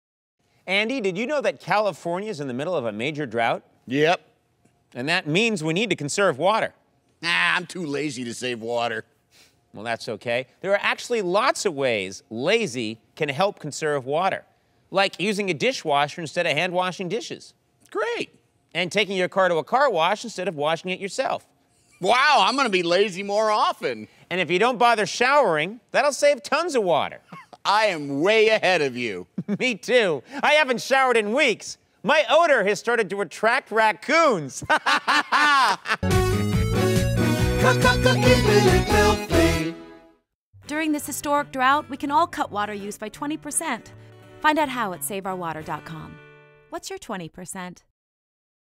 Drought PSA - Conan Lazy
Conan O'Brien and Andy Richter discuss the importance of water conservation during California's historic drought. This and other similar drought-awareness PSAs are being aired across Northern California throughout the summer of 2015 thanks to the generous support of Comcast.